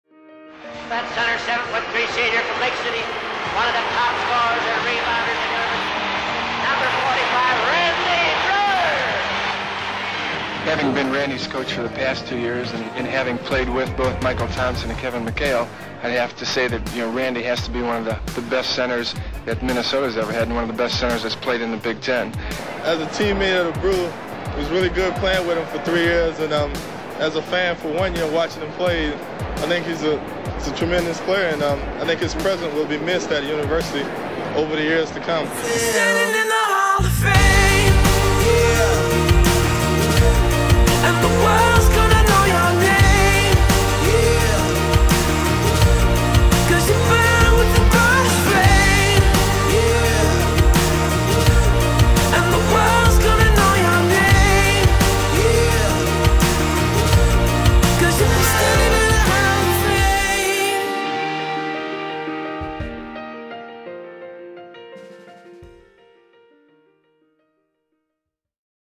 Randy Breuer: Minnesota Gophers Tribute Video
Randy Breuer, a member of the Gophers from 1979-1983, led the Maroon and Gold to a Big Ten Championship title in 1982. The Lake City, Minn., native was voted the team MVP in 1983 after leading the Gophers in scoring, rebounds and blocked shots for the third consecutive season. A two-time All-Big Ten first team selection, Breuer's name still reads in the Minnesota record books as he sits third on the All-Time Leading Scorers list with 1,777 points and second with 229 career blocks. He is one of only five Gophers all-time to record more than 1,000 points, 500 rebounds, and 150 blocks.